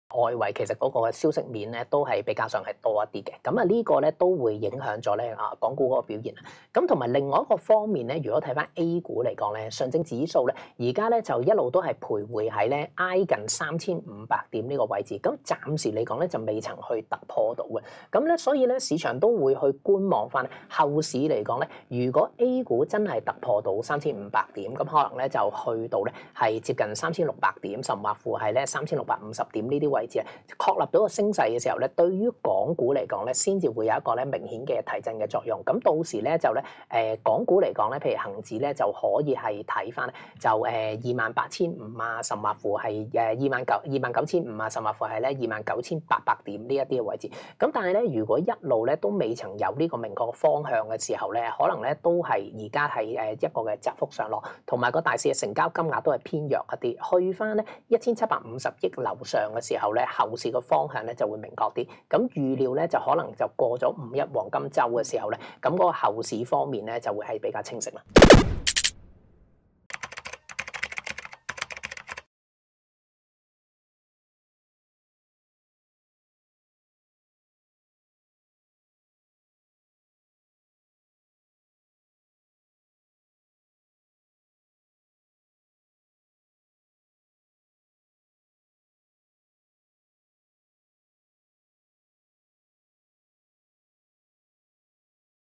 【ET開市直擊】（精華）港股悶局未破 恒指睇咩位聶振邦話你知（2021年4月27日）
恒生指數今早輕微低開後走勢偏軟，在28900點水平窄幅上落。五月將至，適逢內地「五一假期」，恒指睇咩位？華盛證券分析師聶振邦話你知。